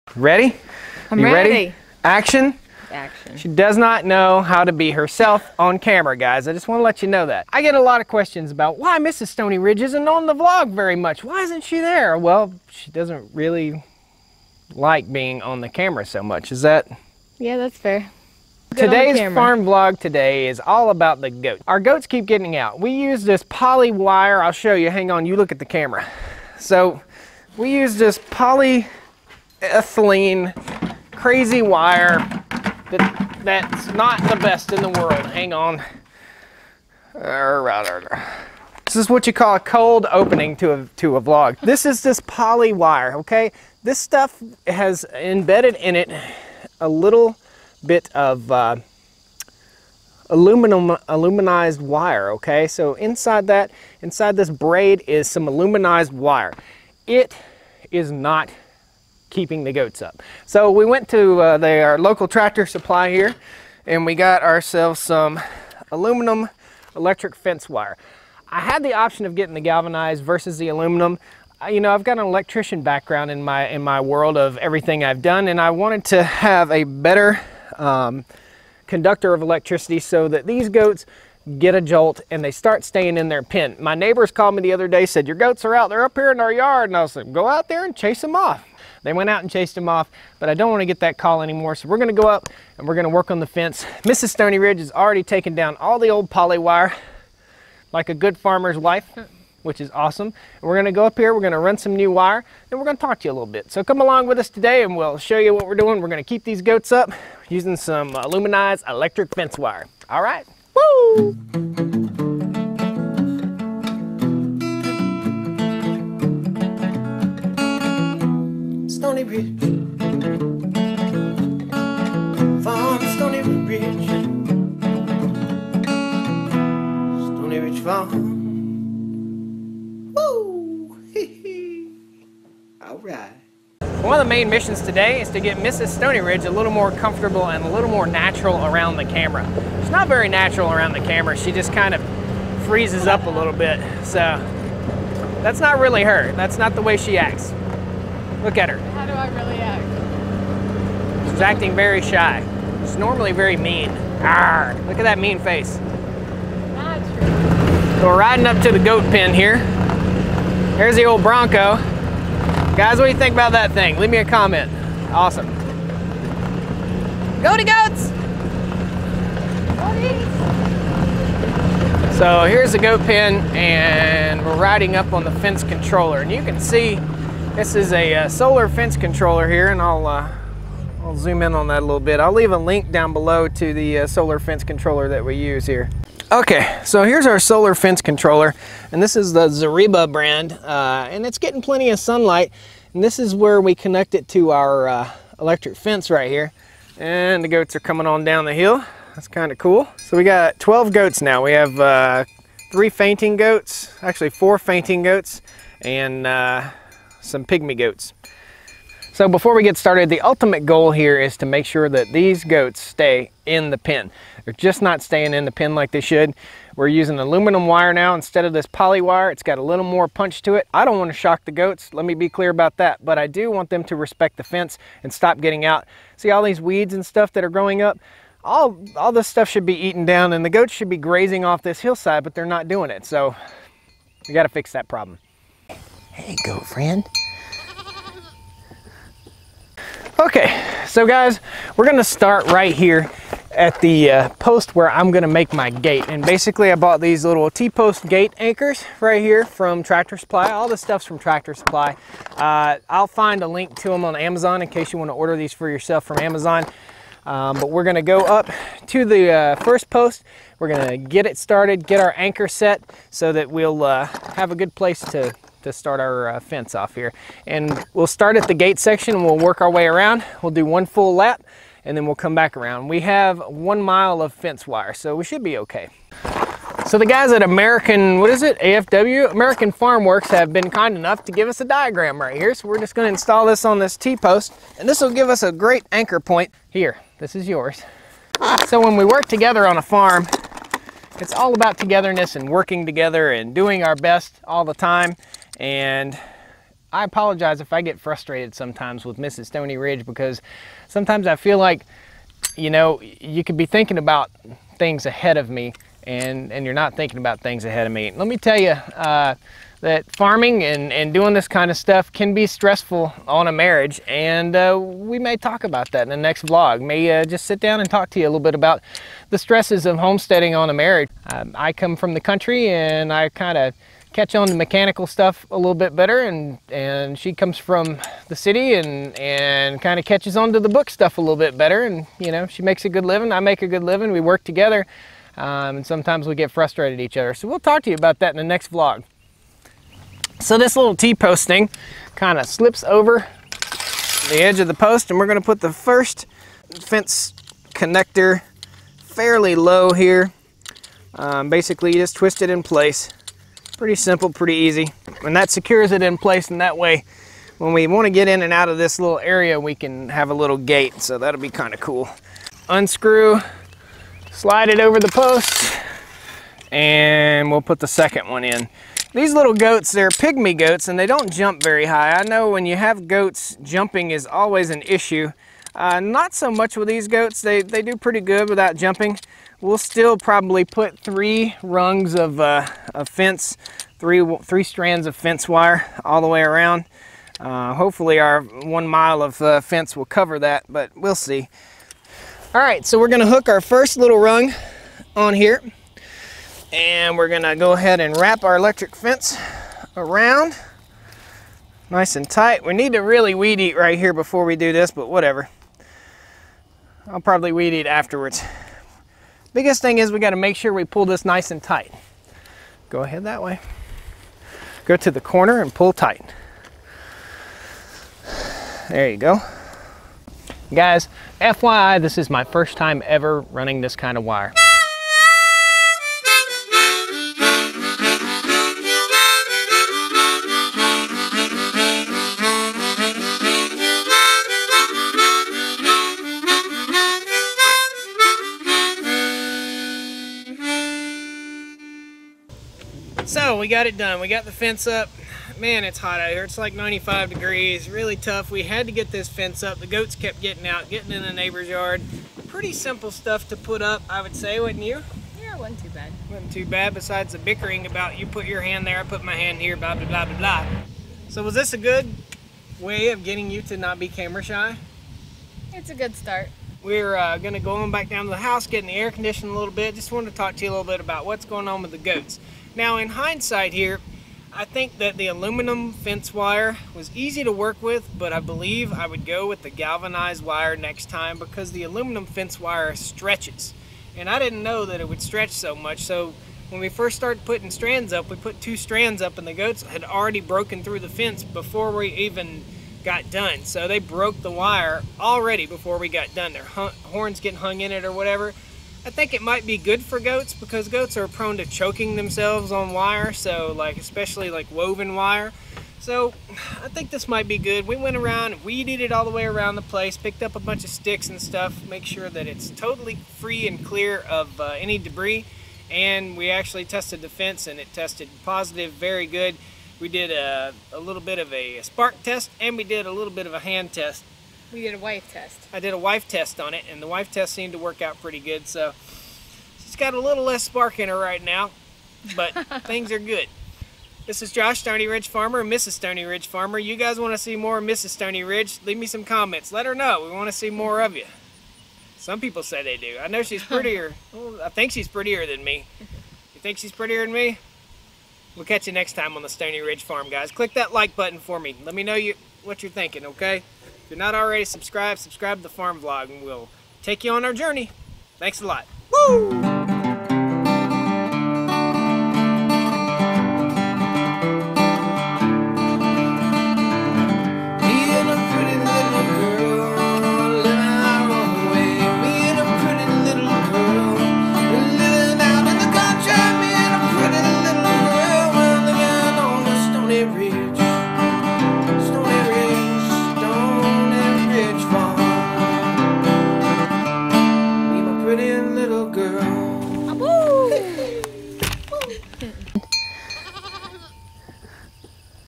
PESKY GOATS WONT STAY IN THE PEN! Fence gets re-worked..Why Mrs StoneyRidge isn't on the Vlog much?
Today's vlog has us fixing the goat fence. The pesky goats dont go far but they keep getting out...they no longer have a respect for the electric fence that keeps them safely penned up. So we are rebuilding it today and talking a bit about Mrs Stoney Ridge and why we dont see much of her on the vlog. Enjoy..here are the links to fencing supplies we used today:

Fencing Stuff:

Zareba Fence Controller Solar

T-Post Gate Clamps

Aluminum Fence Wire